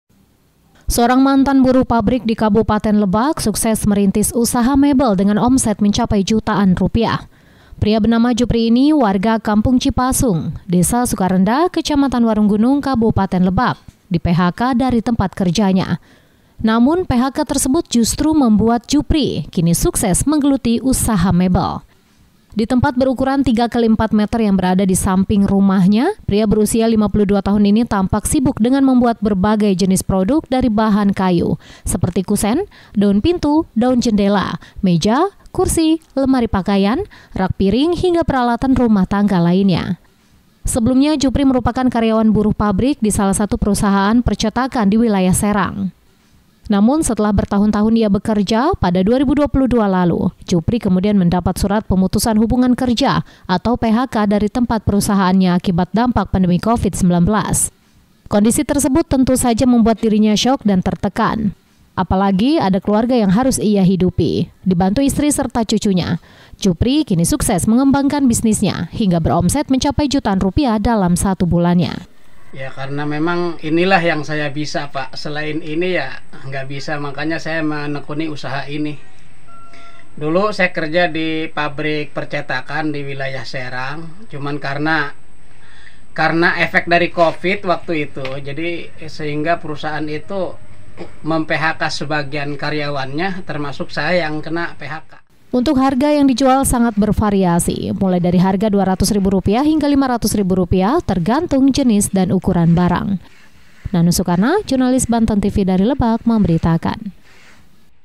MANTAN BURUH PABRIK SUKSES JADI PENGUSAHA MEBEL
Mungkin bagi sejumlah orang, Pemutusan Hubungan Kerja atau PHK menjadi momok yang menakutkan, namun tidak dengan pria yang satu ini, seorang mantan buruh pabrik di Kabupaten Lebak, Banten, sukses merintis usaha mebel dengan omset mencapai jutaan rupiah.